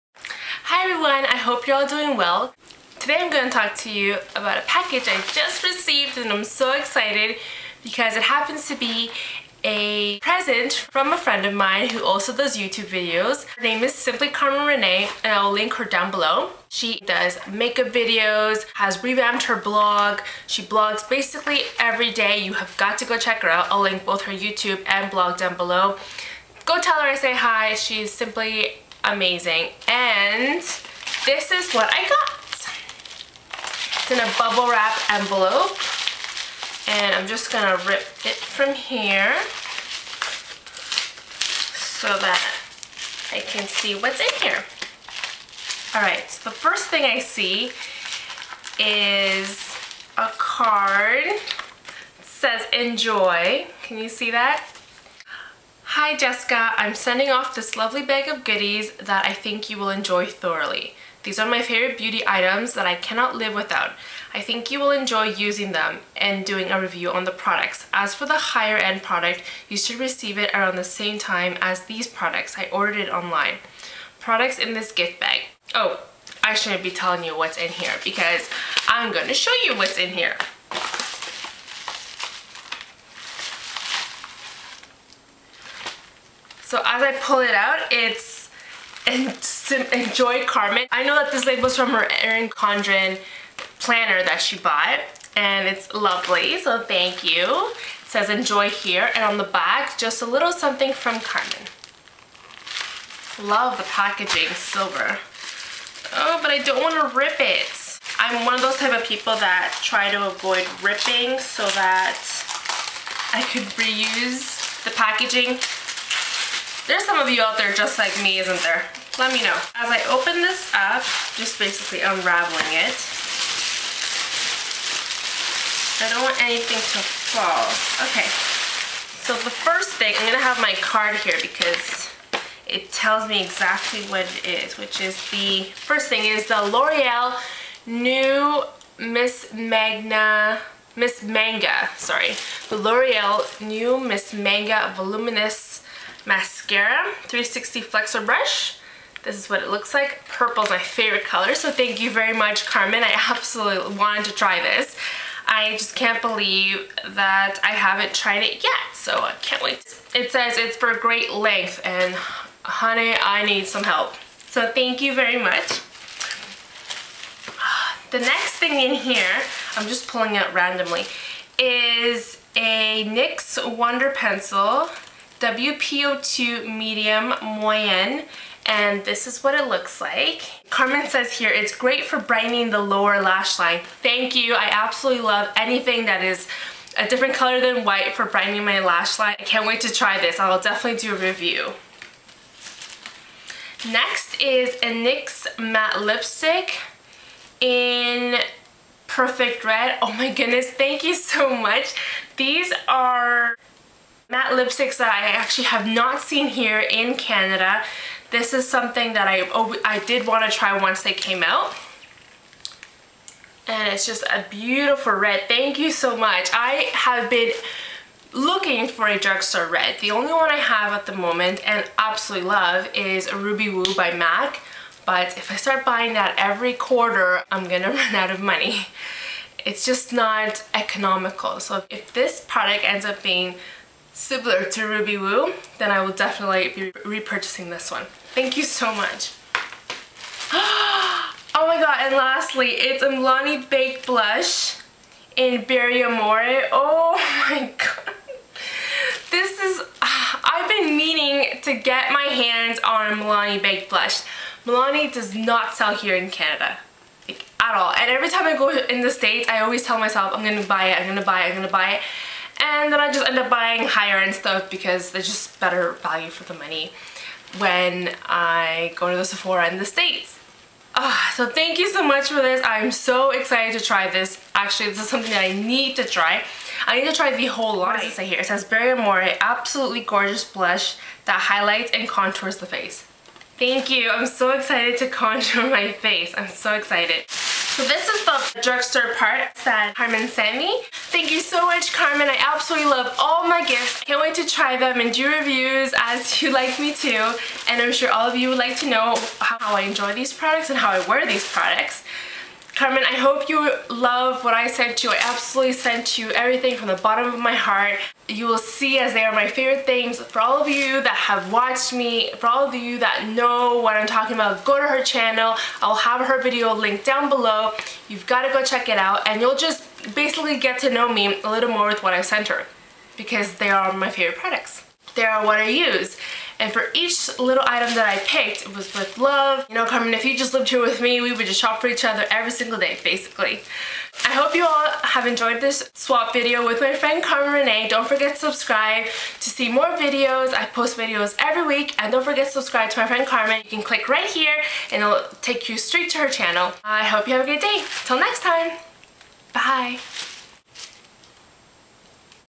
First SWAP || MrsJesskah
Open "Show more" for related information:

I hope you enjoyed my first swap video with Carmen Renee. Thumbs up for swap videos and let me know what other videos you would like to see.
xo,
Jessica

.:. JehVas video